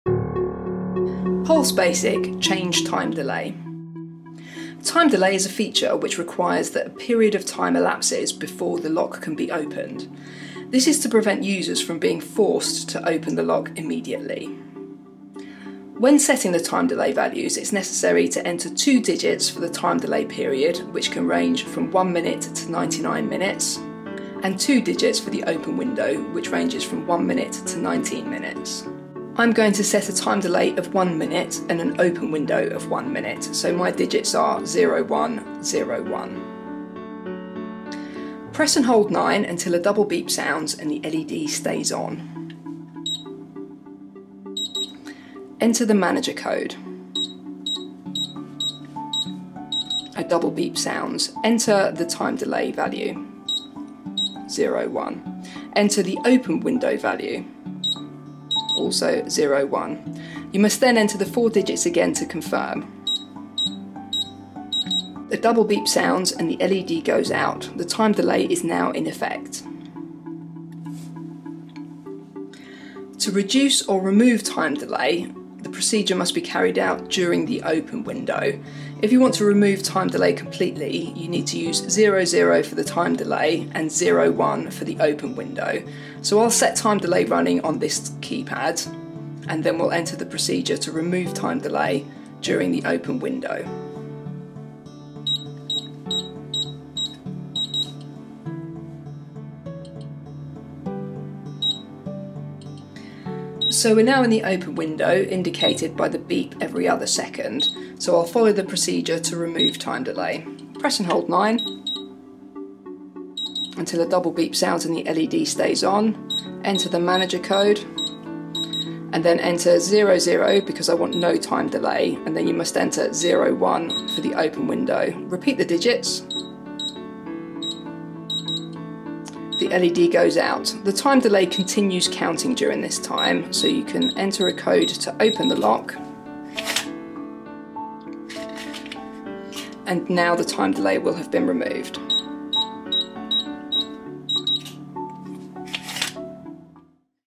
Pulse Basic - Time Delay
A video showing the process to add and remove time delay on a Tecnosicurezza Pulse Basic safe lock.